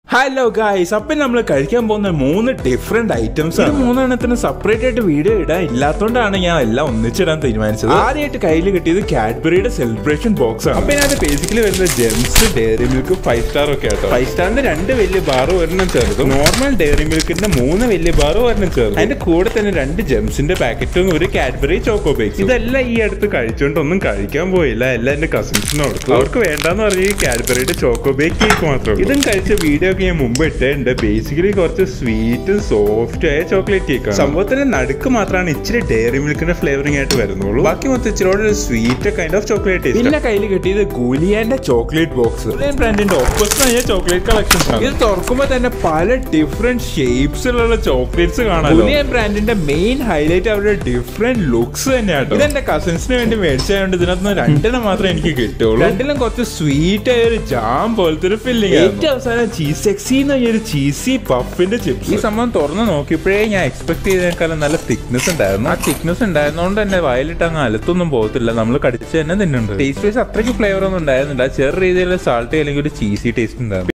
Cadbury Celebration Chocolate, Cheesy Chips & Guylian Opus
Cadbury Celebrations Assorted Chocolate Gift Pack, guylian opus, Miaow Miaow Cheexy. A symphony of flavours, ready to give in a convenient and elegant Gift Wrapped Ballotin. The ballotin is hand wrapped in a royal blue gift wrapping and completed with a stylish golden ribbon and a customizable booklet for a personal message. Making GuyLian chocolates so unique and delicious takes skill, effort and craftsmanship. GuyLian gives chocolate lovers all over the world an unforgettable sensation, to be savoured slowly.Each ballotin contains 16 delicacies inspired by classical masterpieces and operas.

💲 PRICE
Cadbury Celebrations ₹182
Guylian ₹1,419